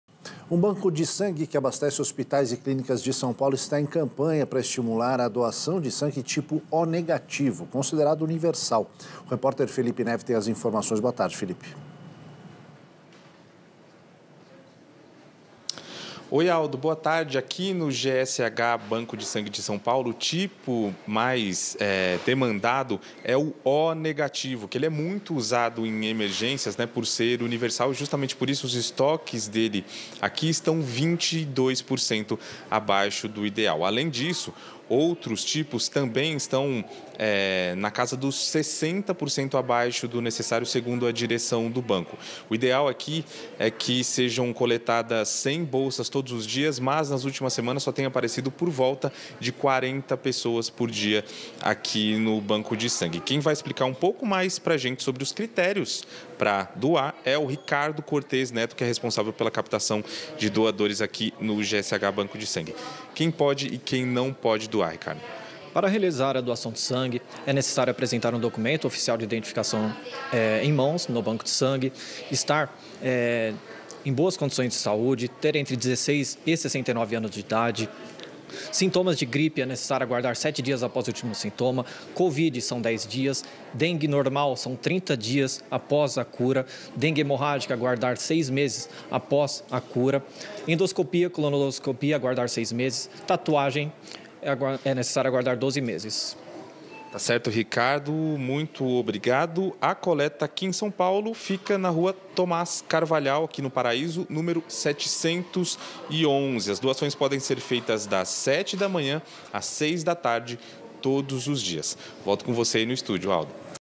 Banco de Sangue em São Paulo lança campanha para aumentar doações do tipo O negativo
Um banco de sangue que abastece hospitais e clínicas de São Paulo está em campanha para estimular a doação de sangue tipo O negativo, considerado universal.

O tipo sanguíneo que está em baixa aqui é o O negativo. Ele é muito usado durante emergências por ser universal. Justamente por isso, os estoques estão 22% menores.

Além disso, as doações gerais, de outros tipos, estão até 70% abaixo do necessário, de acordo com a direção do banco. O banco precisa, em média, de 100 bolsas novas por dia. Mas só estão aparecendo cerca de quarenta a cinquenta pessoas por aqui.

A coleta em São Paulo fica na Rua Tomás Carvalhal, 711, no Paraíso. As doações podem ser feitas das sete da manhã às seis da tarde.